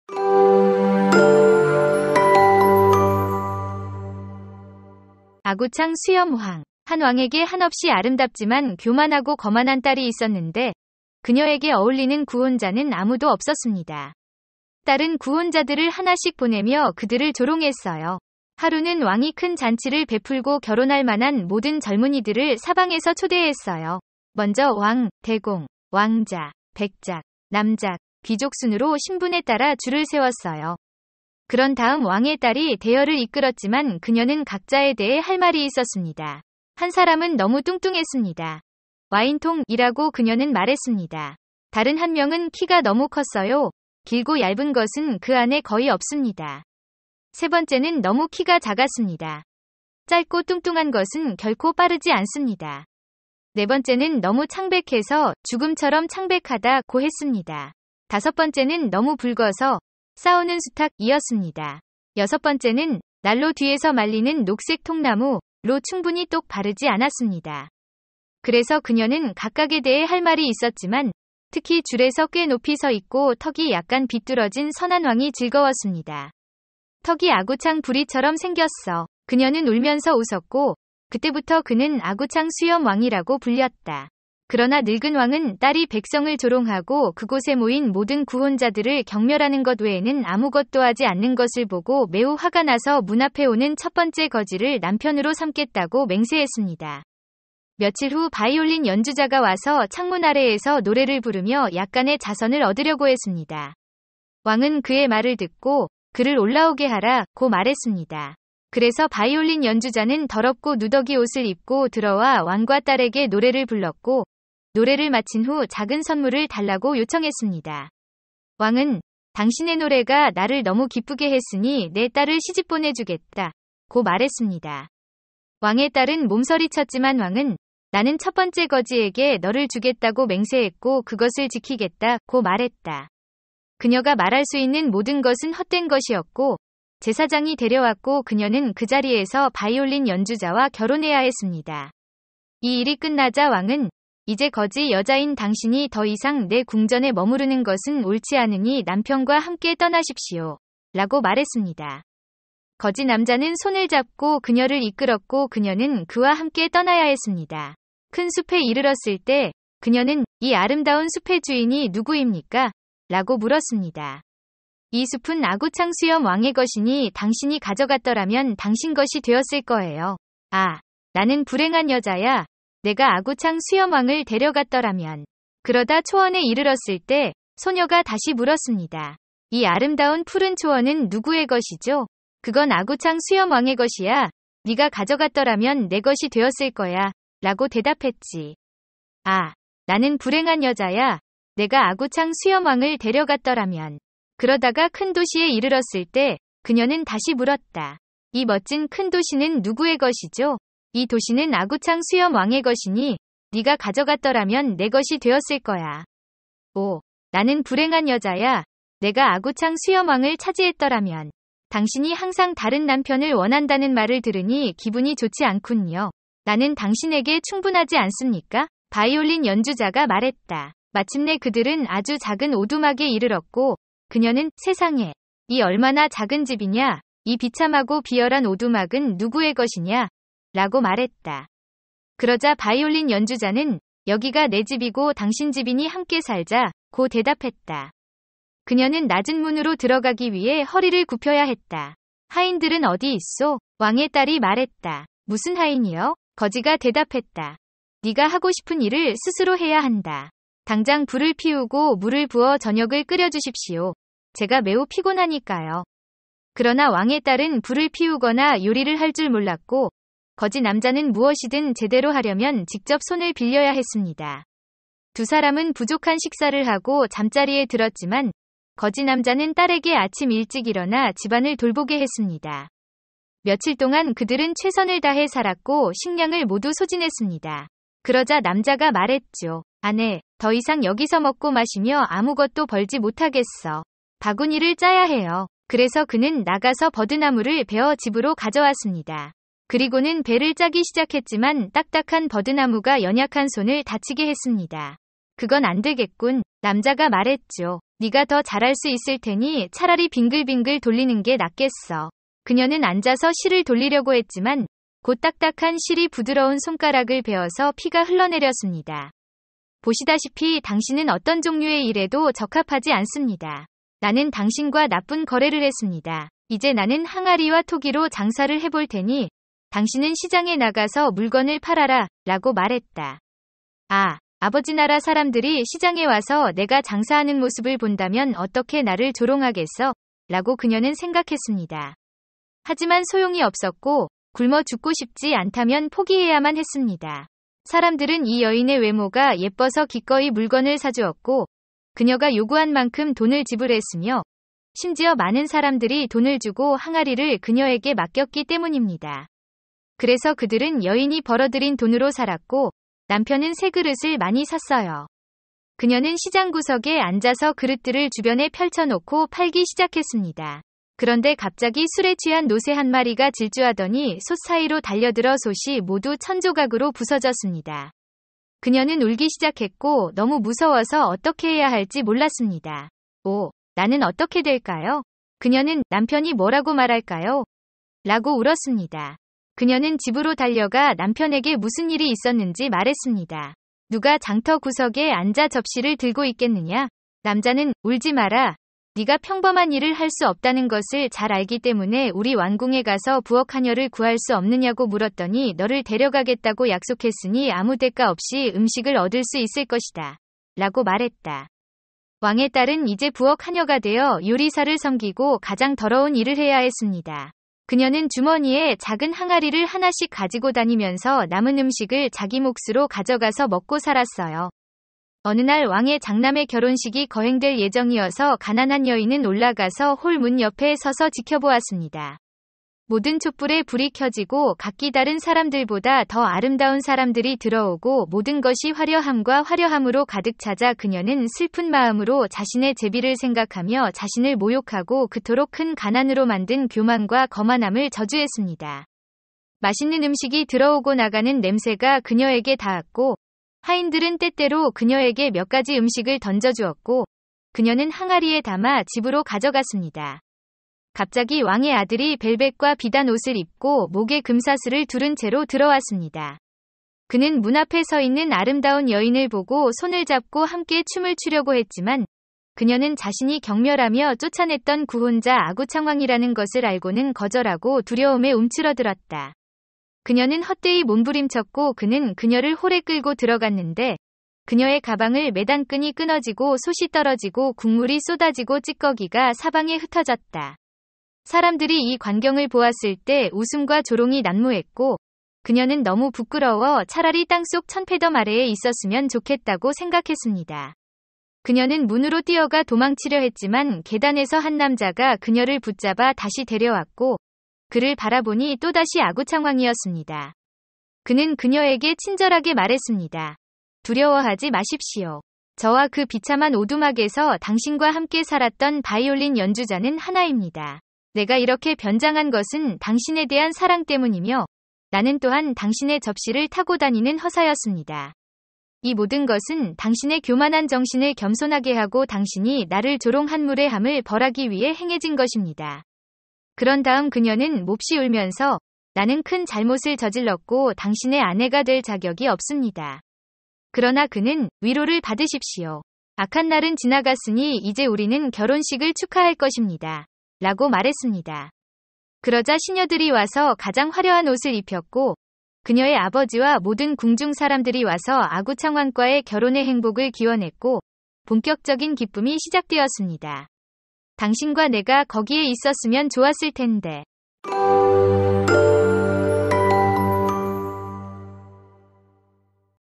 👑🧔 자존심 강한 공주, 자비로운 왕- '아구창 수염 왕'으로 떠나는 여행! | 고전 동화의 재창조
'아구창 수염 왕'에서는 파트너를 고르기엔 너무 교만한 공주를 만나게 돼요! 그래서 아빠는 '노크하는 첫 번째 거지가 잭팟을 터뜨린다'고 선언해요! 윙크와 함께 바이올린 연주자가 등장하고, 곧이어 왕실 여인의 인생이 뒤바뀌어요. 궁전부터 프라이팬까지, 공주는 옛 집에서 설거지를 하다가 왕실 파티에 참석한 아저씨 수염 왕에게 발각됩니다. 놀랍군요! 왕관을 쓴 바이올린 연주자예요! 이 교활한 왕의 게임? 거만한 자를 낮추는 거죠. 이제 자존심이 완전히 다듬어진 주인공은 왕과 다시 만나 노을 지는 도시로 춤을 추며 떠납니다. 자존심은 한풀 꺾이고, 사랑이 지배하며, 모두가 인생의 작은 아이러니에 웃음을 터뜨리는 이야기입니다.